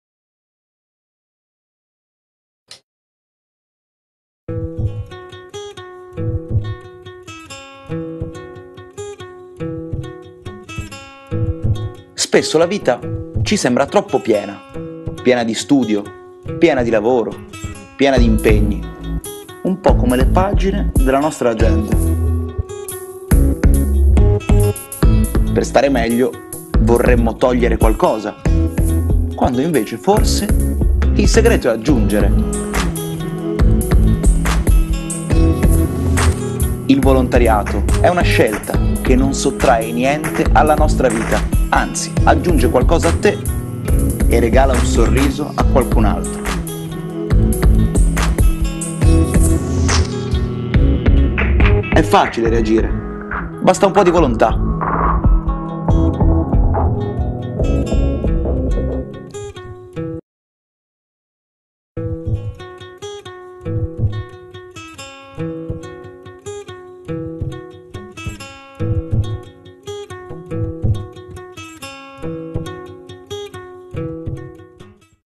Take Action 2013 Agata Matteucci
Lo spot realizzato dalla studentessa Agata Matteucci e vincitore del concorso Take...Action 2013 promosso dall'AIL.